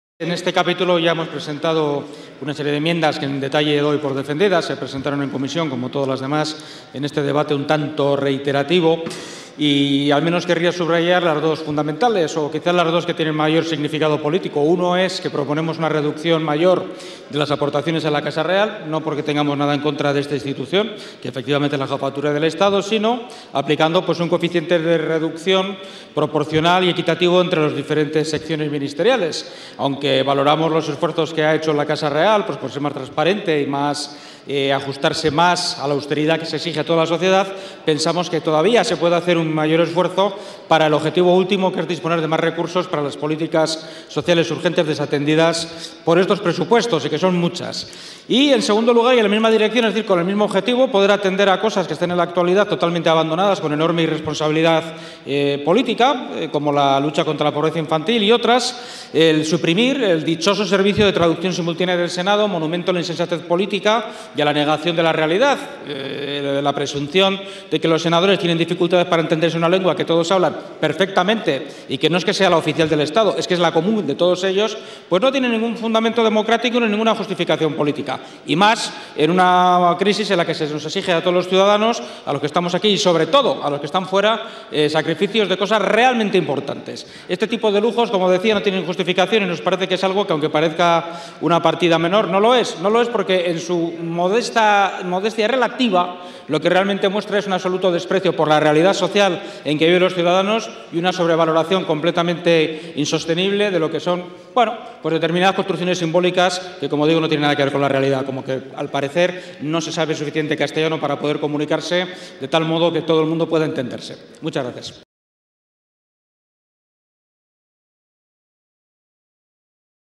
Gorriarán (UPyD): "No se pueden mantener partidas de lujo y recortar en servicios sociales"
El diputado magenta afirma que no se puede recortar en partidas como servicios sociales o educación y mantener "partidas de lujo". Por lo que propone la reducción de las aportaciones de la Casa Real y la eliminación del uso de pinganillos en el Senado.

Intervención de Carlos Martínez Gorriarán, diputado de Unión Progreso y Democracia, sobre el debate de los Presupuestos Generales del Estado para el año 2015 sobre las secciones del 1 al 5 (Casa de S.M. el Rey/Cortes Generales/T. de Cuentas/T. Constitucional/Consejo de Estado) y 8 (CGPJ).

Pleno 11/11/2014